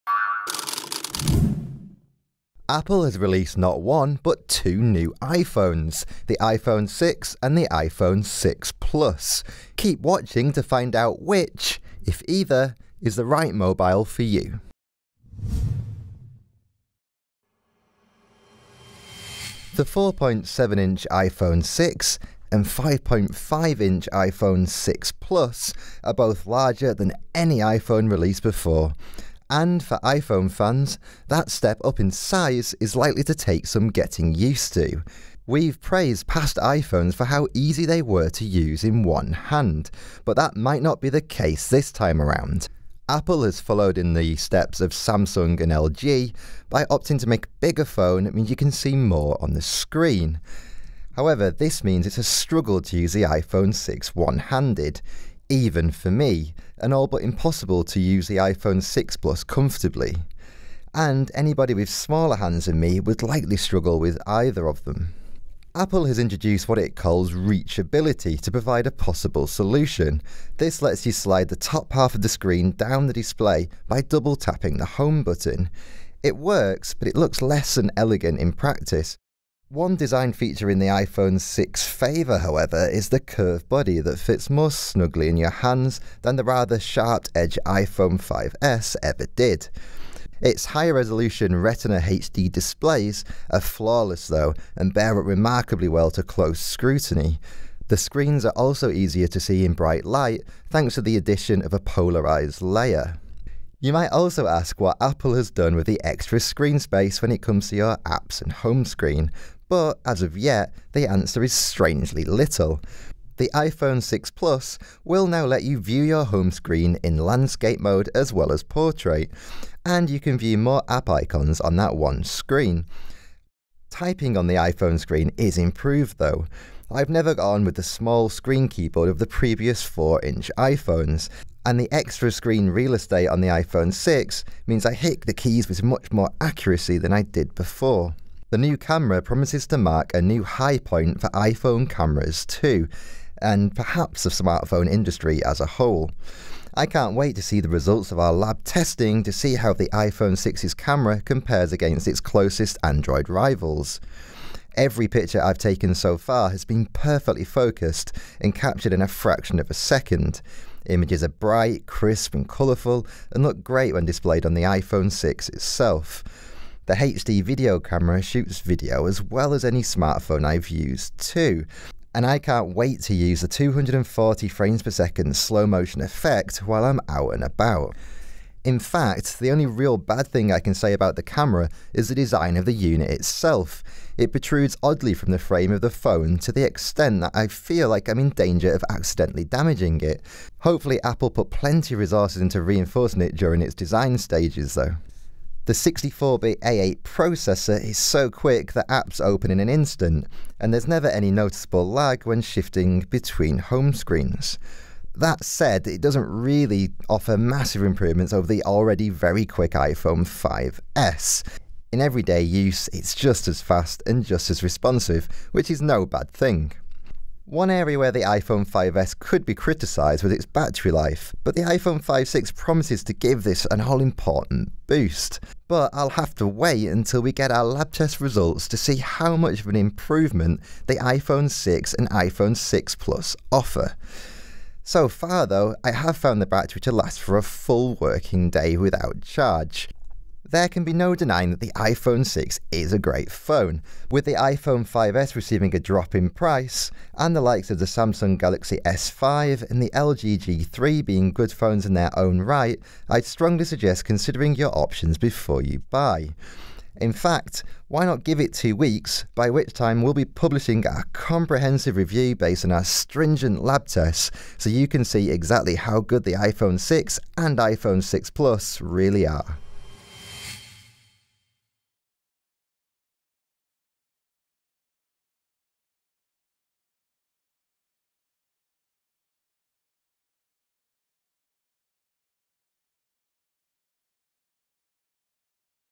Apple iPhone 6 and 6 Plus - Which? first look
Apple has released the iPhone 6 and iPhone 6 Plus – each larger than any iPhone released before with 4.7 and 5.5-inch displays respectively.

Each iPhone 6 is equipped with a faster processor, the new higher resolution Retina HD screens, and the latest operating system in iOS 8. Watch on as we find out how those larger displays change the way you use your iPhone, take some shots with the improved camera and find out where there’s still room for improvement.